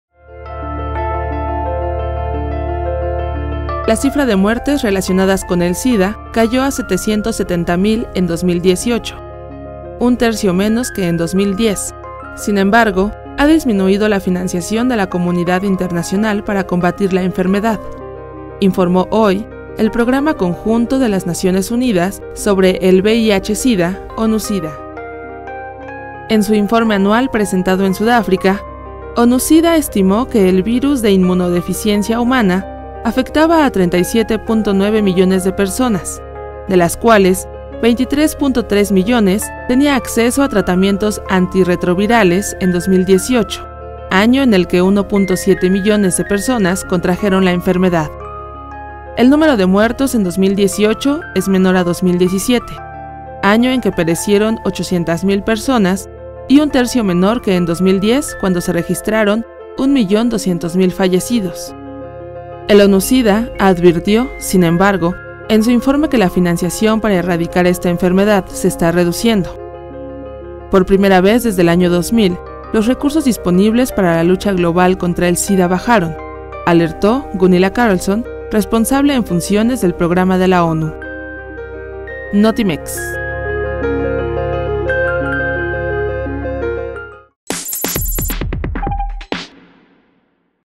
Más de 700 mil personas murieron en el mundo por Sida en 2018: ONU
En 2018 1.7 millones de personas contrajeron la enfermedad